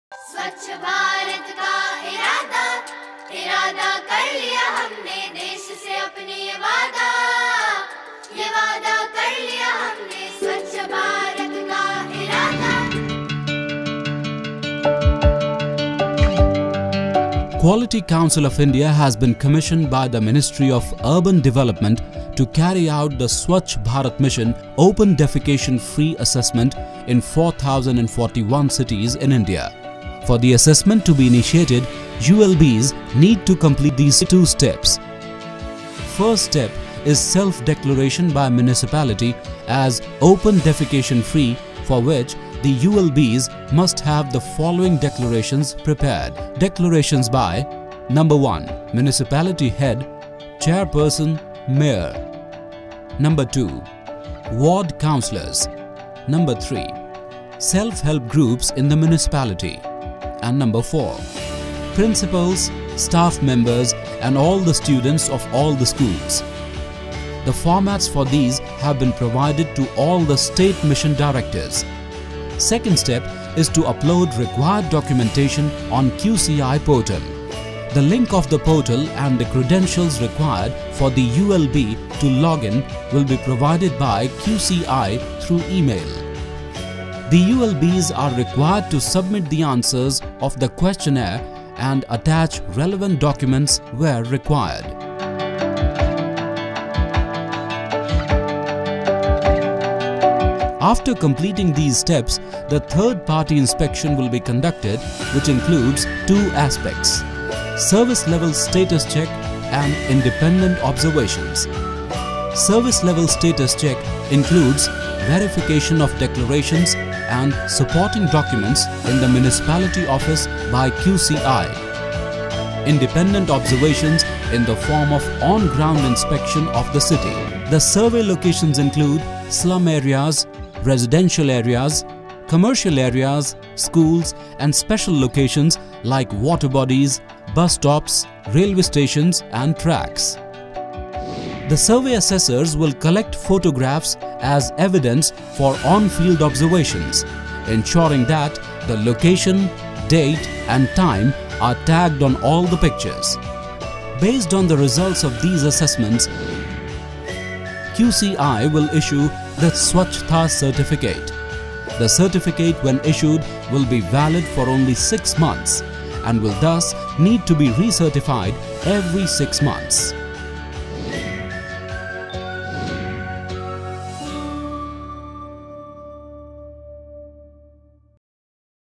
How to get declared as an Open defecation free Urban Local Body?
Quality Council of India (QCI) has been commissioned by the Ministry of Urban Development to carry out the Swachh Bharat Mission-Open Defecation Free Assessment in 4041 cities in India.

For the assessment to be initiated ULBs need to completed these two steps

First step is Self-Declaration by Municipality as Open Defecation Free

Second step is to upload required documentation on QCI Portal.

The link of the portal and the login ID and Password required for the ULB to login will be provided by QCI through E-Mail. The ULBs are required to submit the answers of the questionnaire and attach relevant documents where required.

After completing these steps the Third Party Inspection will be conducted which includes two aspects, Document Check and On-Ground Observations.

Based on the results of these assessments, QCI will issue Swachhata certificate.

The certificate when issued will be valid for only 6 months and will thus need to be re-certified every 6 months.